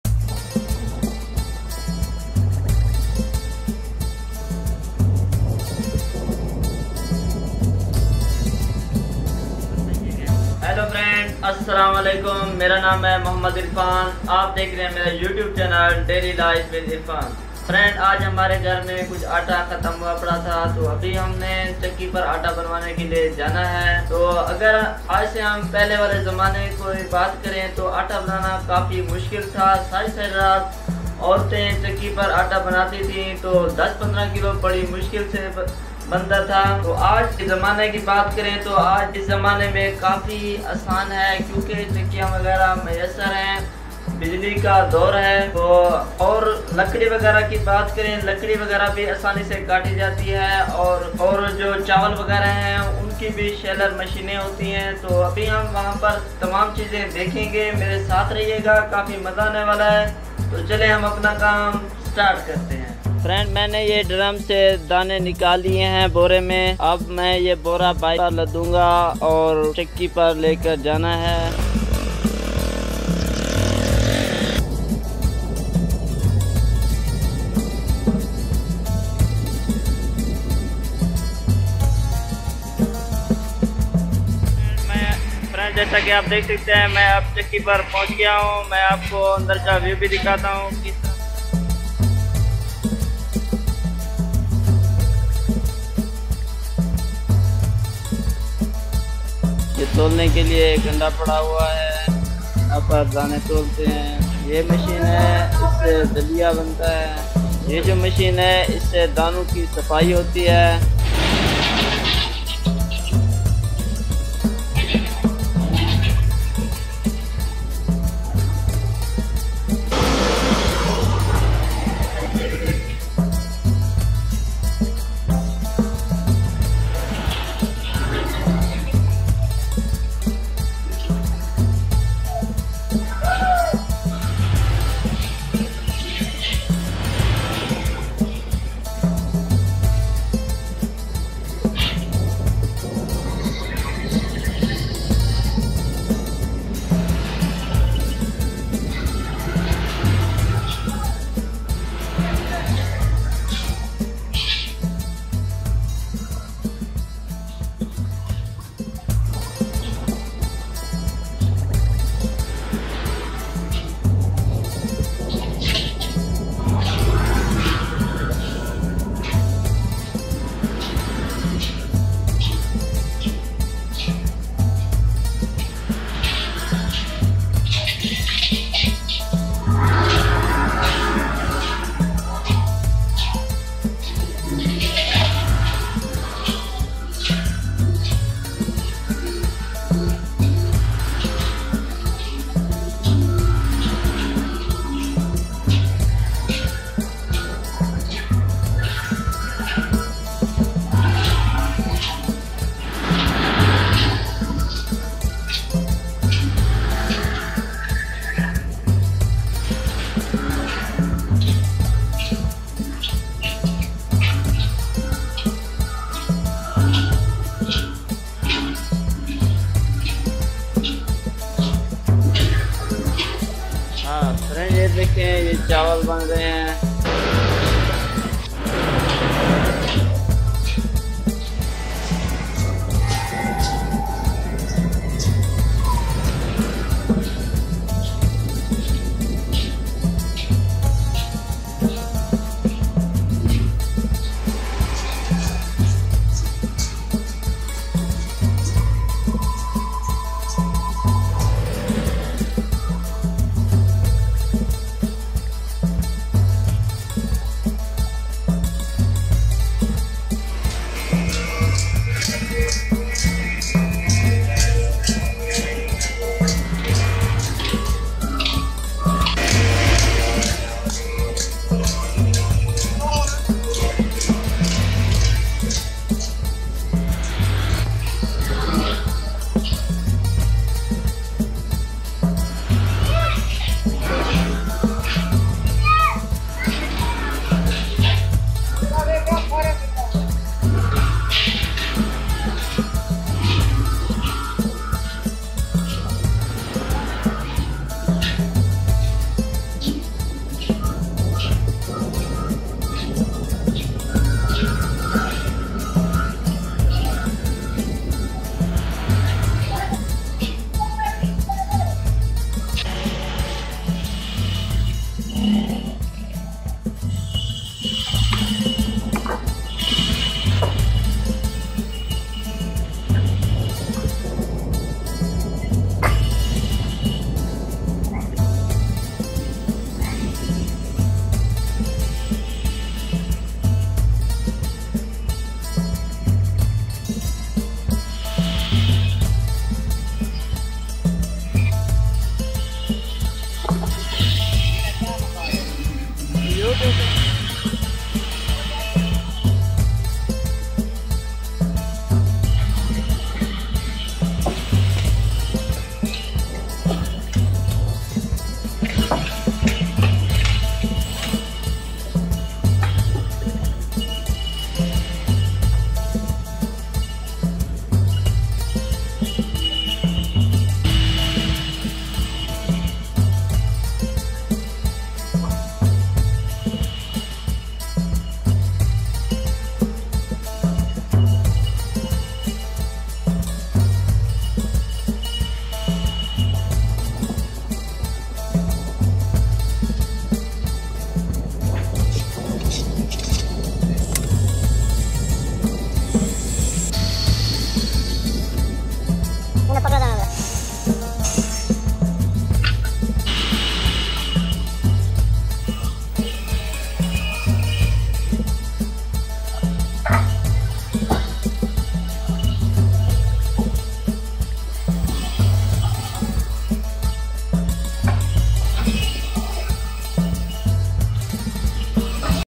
Difference Between Old Days And Current Days - Rice Shaler floor And Cotton Latest Machinery
AoA! Friends umeed krta ap sb thk hn gy is video main hm deakhien gy phly wly zamany main logo ko kia mushkalat thi or ab kia saholat mayasser hy is video rice shaler machine wood ara machine cotton machine or faloor machine deakhao ga agr video achi lgy video ko subscribe krien share krien like krien or comment krien shukriya